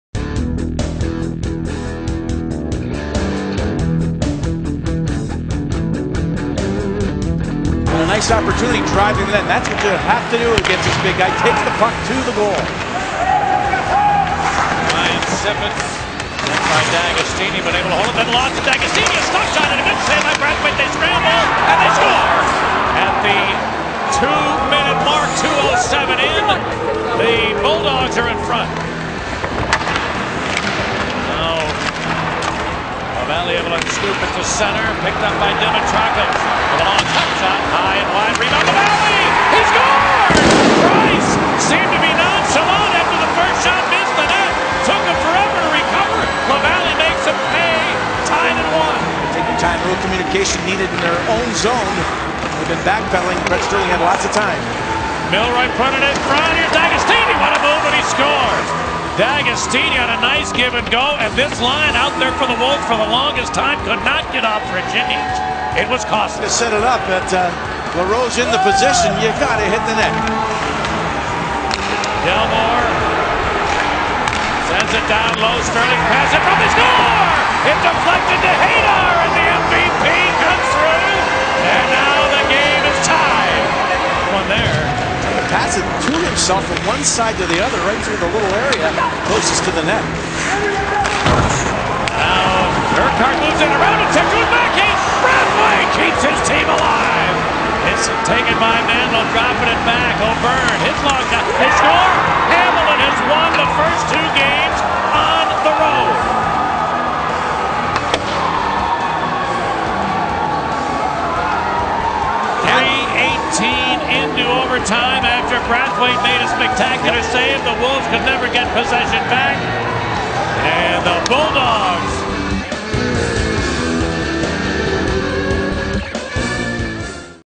Chicago Wolves 2007 Playoff Highlights
2007 Calder Cup Playoffs
Western Conference Final -- GAME 2
Hamilton(3) vs. Chicago(2) OT
05.20.07
Allstate Arena